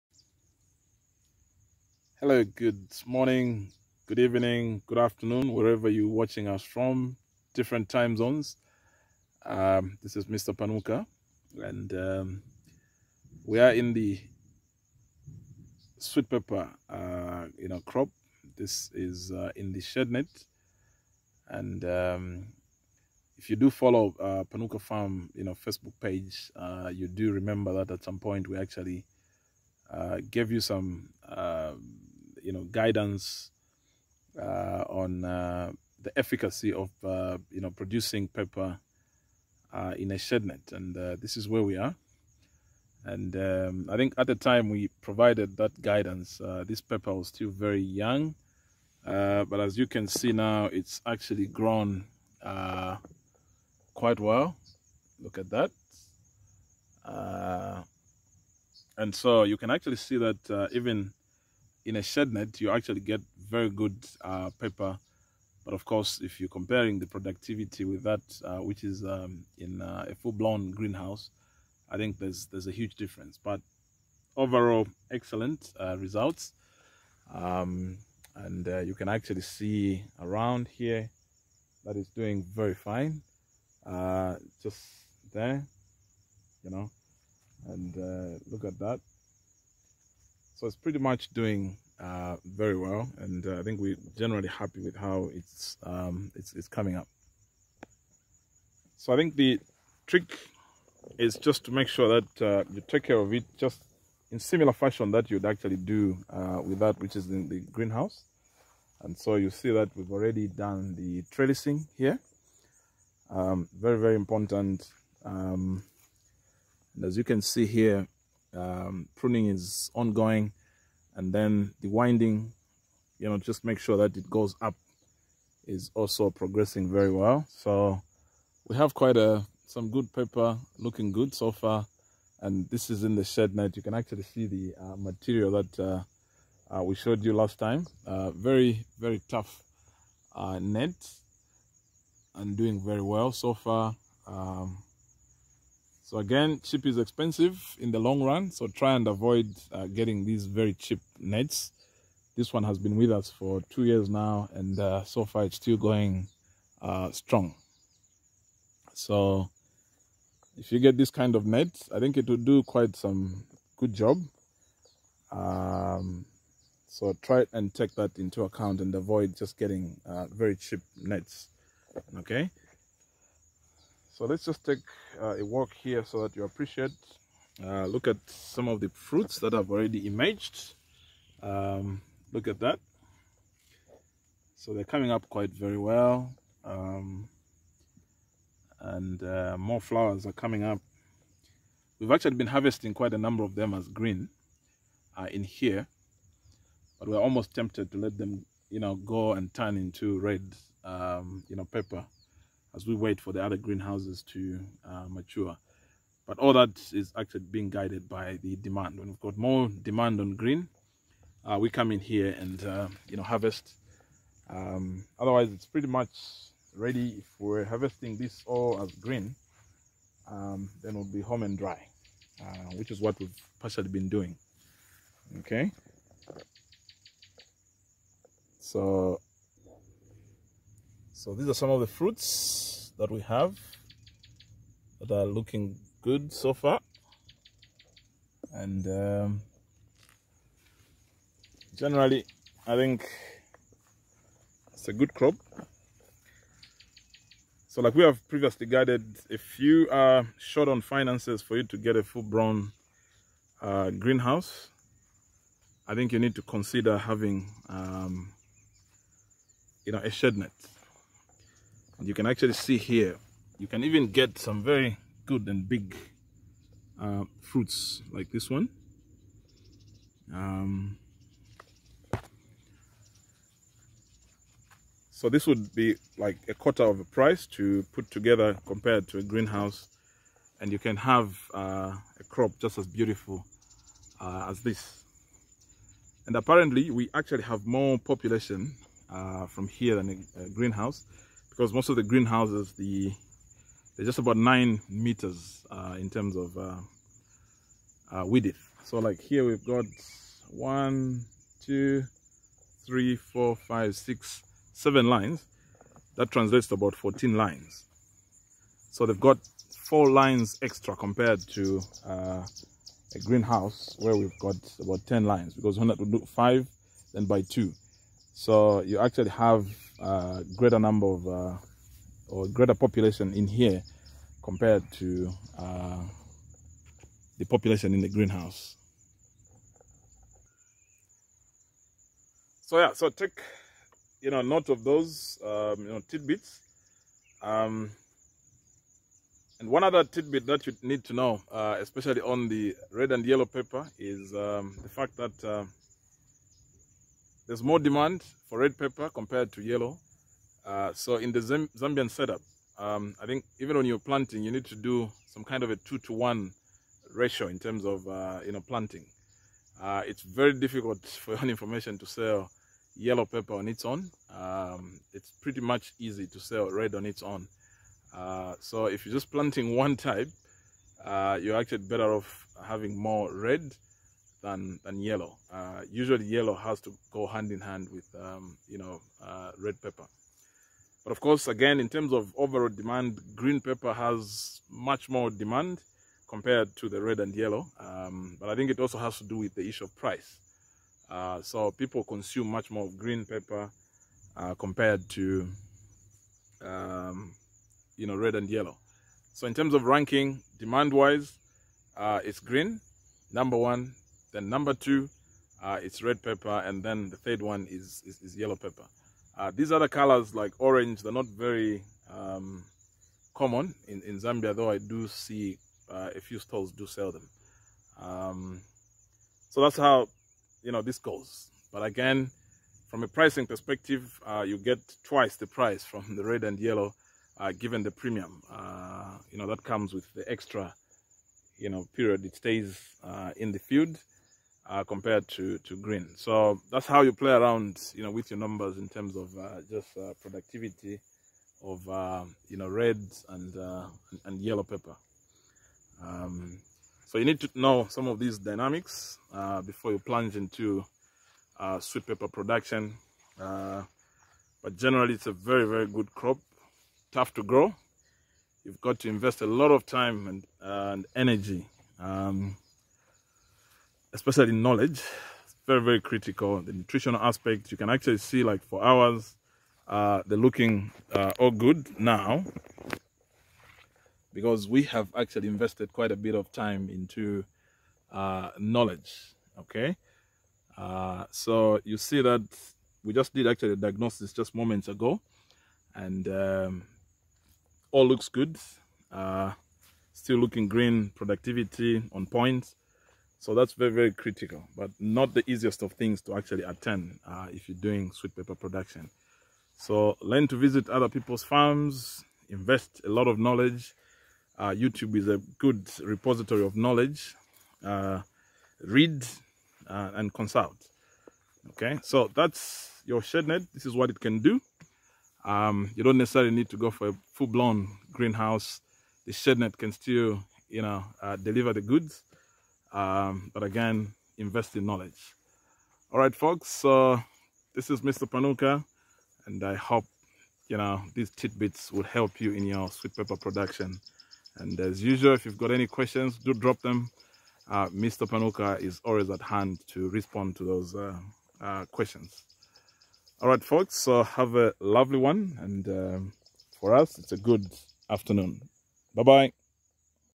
Shadenet - a good option for Sweet Pepper Production
In this video, we demonstrate that a shadenet can still be a good alternative to a greenhouse given that it still provides a much better production environment compared to open field production.